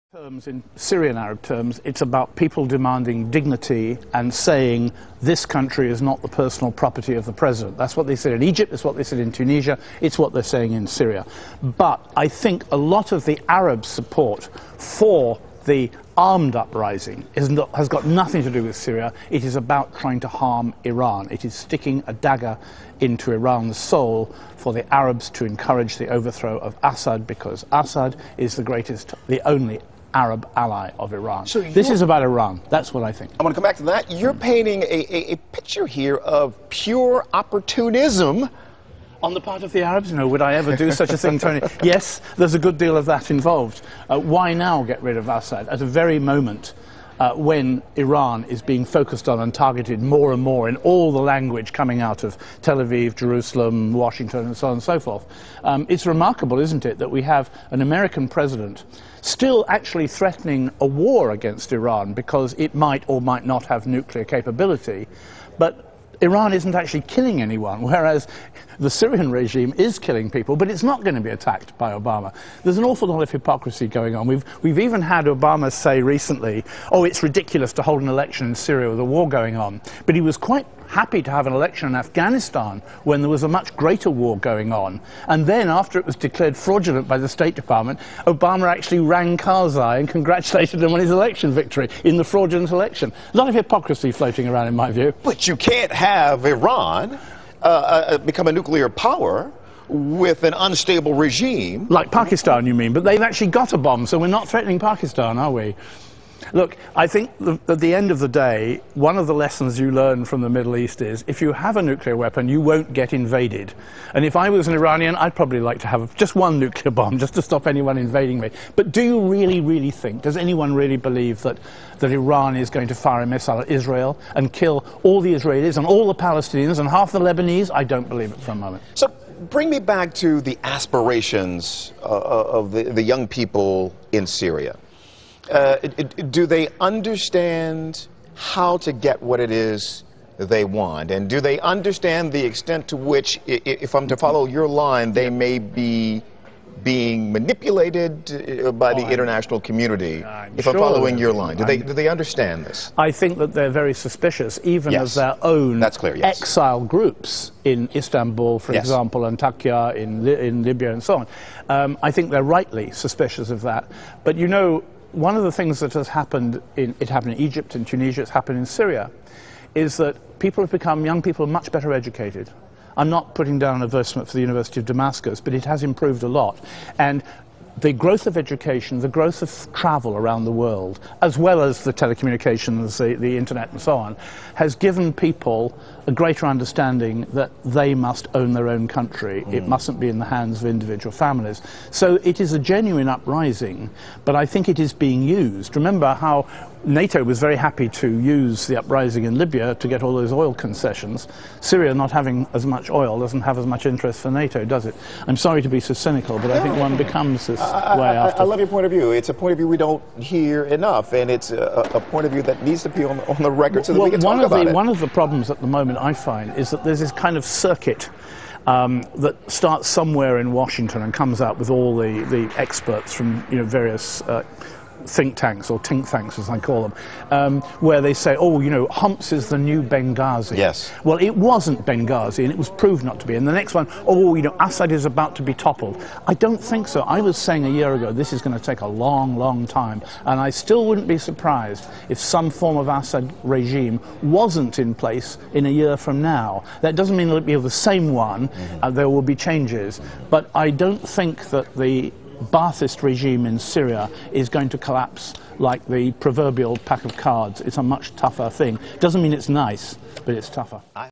Robert Fisk - Iran knows that you won't get invaded if you have a nuclear weapon
Robert Fisk talks to Al Jazeera about the Syrian Civil War. He also talks about the wider Geo Politics at play.

All the right for this video belong to Al Jazeera.